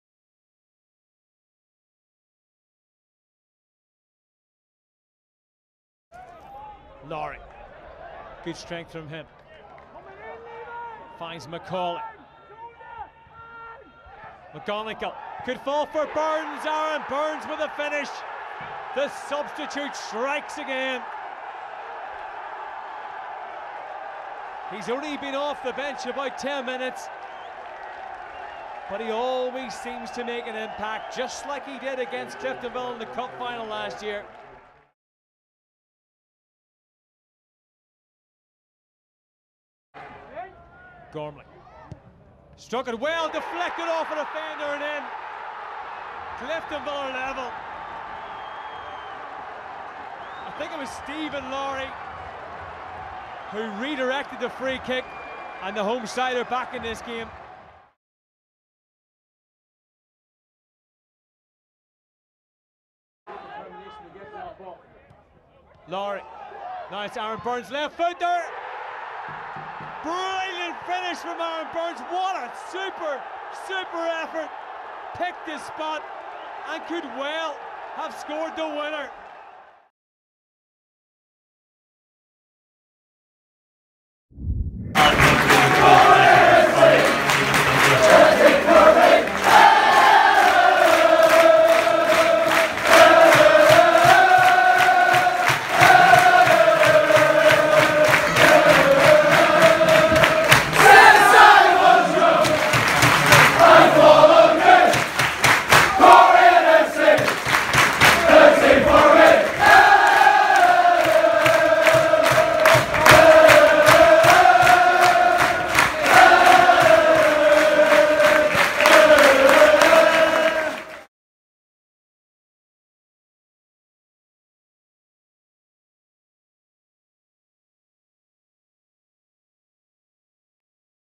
Coleraine vs Cliftonville: Goals
All the goals from our very exciting, 2-1 win against Cliftonville! CFCBible🔵⚪️